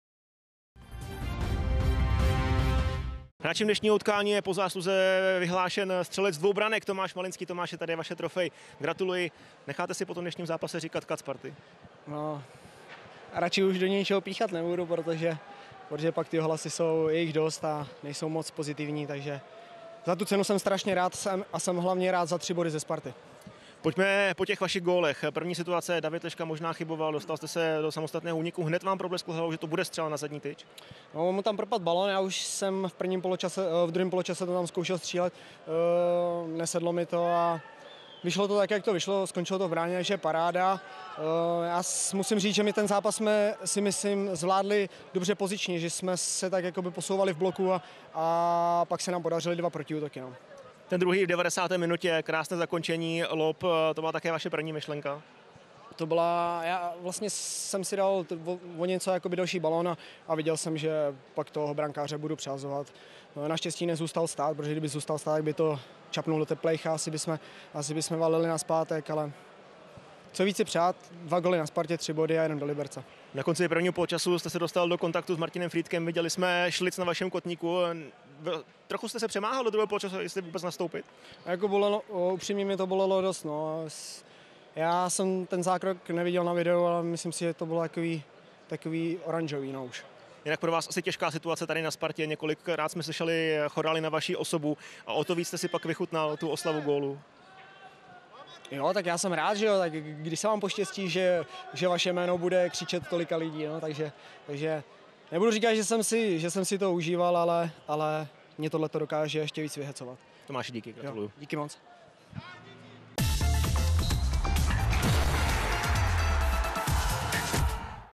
Střelec Malinský: Dva góly na Spartě, tři body. Co víc si přát?
Hvězda souboje Liberce se Spartou? Rozhodně Tomáš Malinský, který se postaral o oba góly Severočechů. "Zápas jsme zvládli dobře pozičně, posouvali jsme se v bloku a pak se nám podařily dva protiútoky," pochvaloval si.